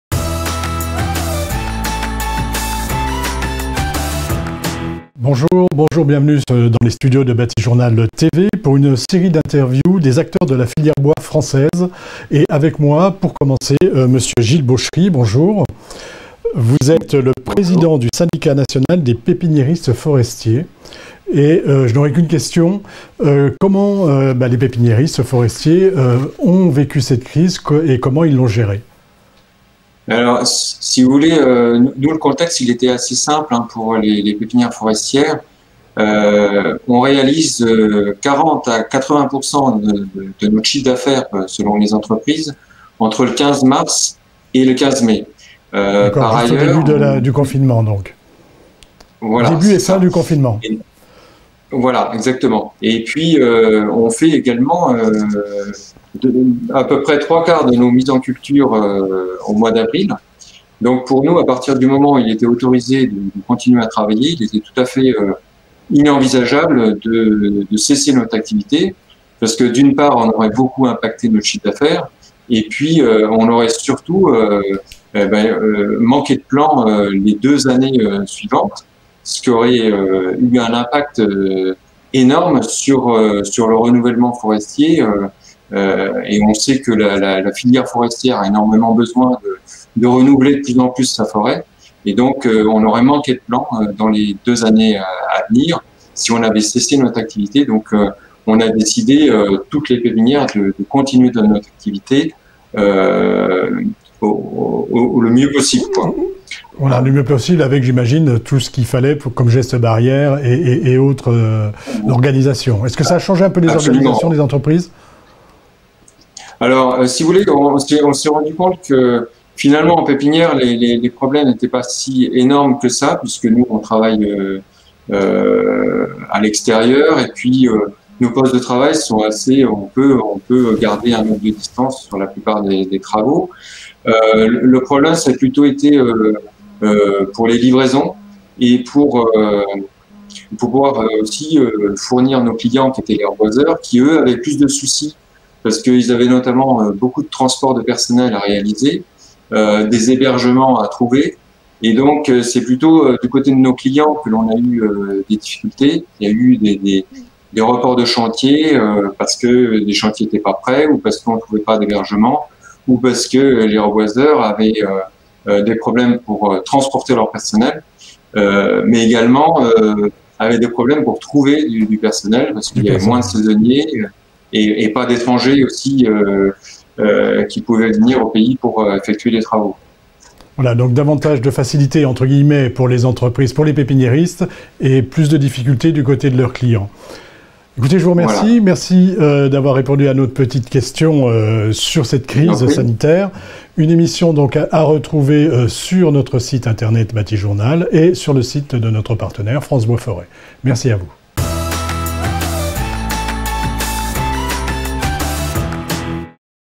Interview de Gilles Bauchery, Président du Syndicat National des Pépiniéristes Forestiers - Part.1
Interview de Gilles Bauchery, Président du Syndicat National des Pépiniéristes Forestiers le 22/06/2020

« Nous avons décidé de continuer notre activité le mieux possible. Nos principaux problèmes pendant la crise concernaient les livraisons et les reports de chantiers. »